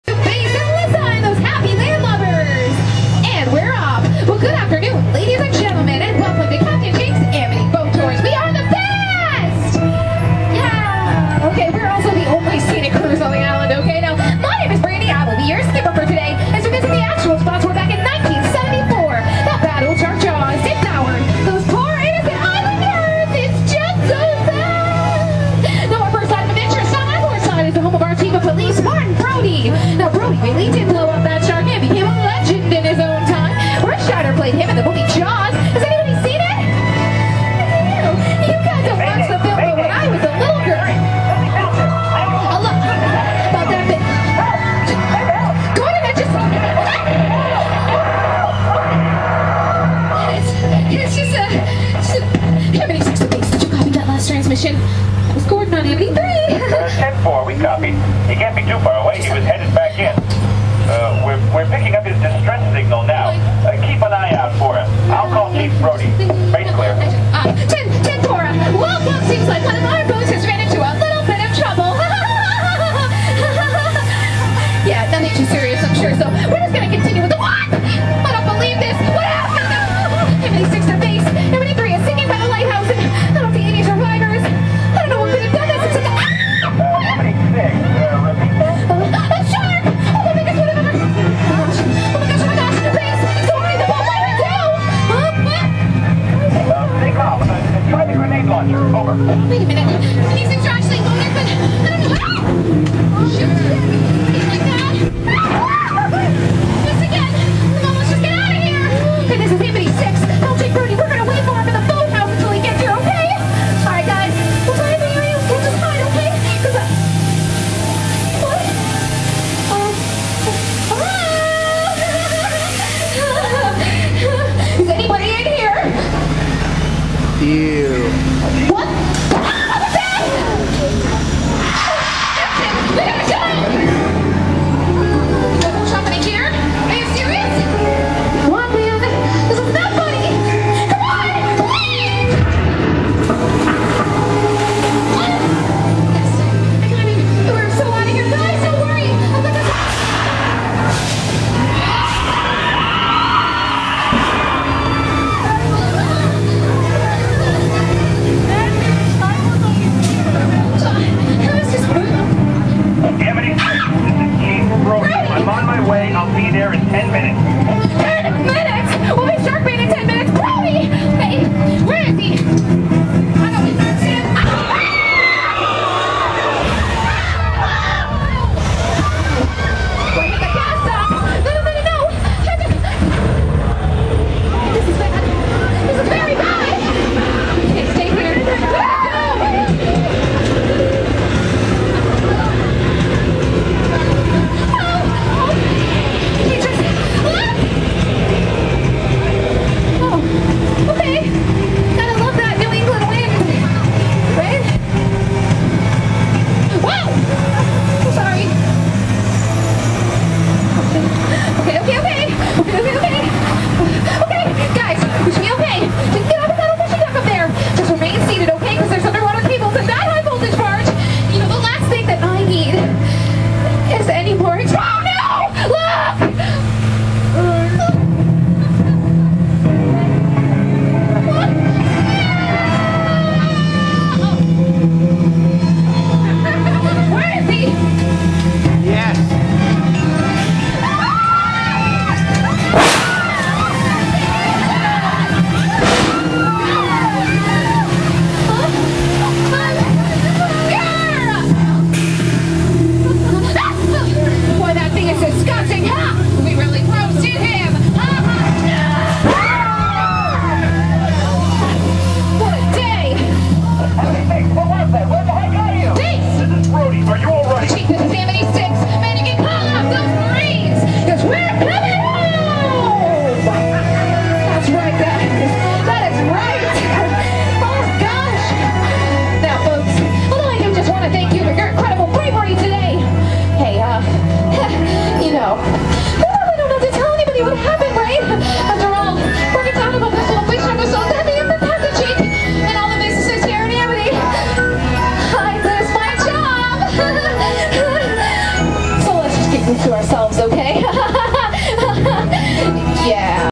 JAWS
This is another of my son's favorite rides at Universal Studios.  The first time he rode this he was only six weeks old.  He is now nearly 15.  He did his best trying to capture all of the shark events, although in my opinion he tends to jump and pan a bit too quickly (don't tell him I said that).  Also, the next to last shark (the one that bites the wire just before "bacon jaws") was not working, so he was very disappointed and will probably be going back to try to get "the perfect video".  In the mean time, here's his first attempt.  Update: He did go back for another video.  Unfortunately, the next to last shark was STILL not working!  For more information on this attraction, check out Universal's web